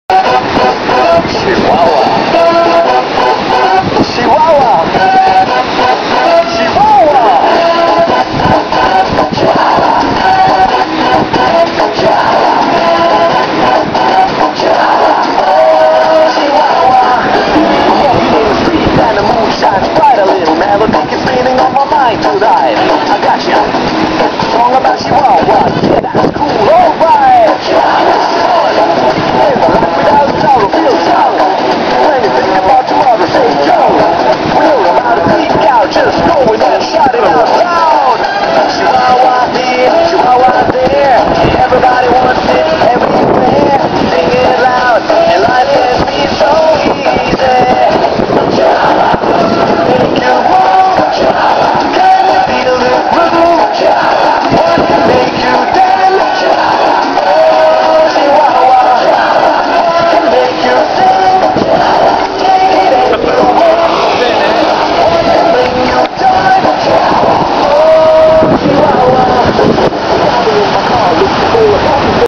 Carlo, il 70enne che balla in spiaggia a Santo Spirito
Il 70enne Carlo che ogni giorno balla in spiaggia davanti ai bagnanti di Santo Spirito.